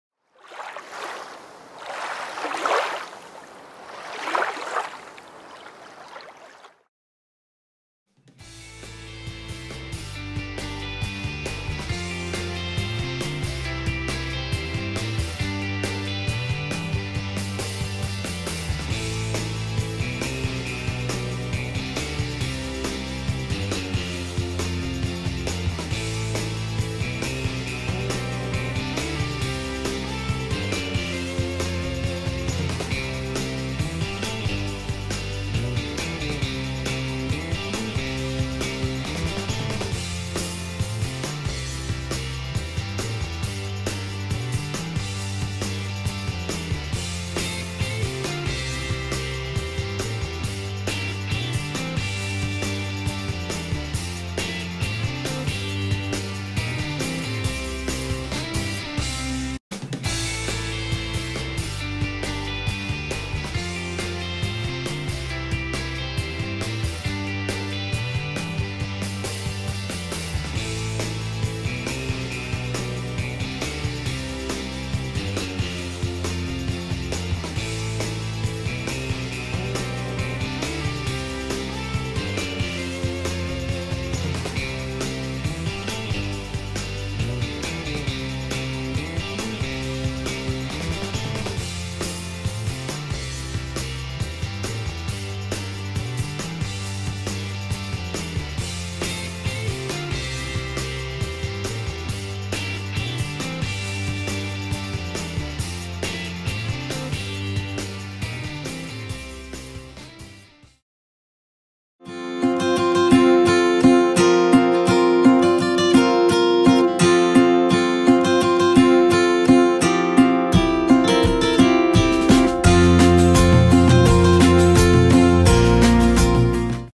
Lippus lures - Minnow artigianale 7cm galleggiante in balsa
Dopo aver realizzto un minnow 7cm galleggiante in samba, di cui ho mostrato come costruirlo in varie live, ho realizzato lo stesso minnow, ma con legno di balsa, con fasi costruttive completamente diverse, pur avendo la stessa sagoma e dimensioni.
A breve uscirà un video tutorial dove mostro come costruirlo.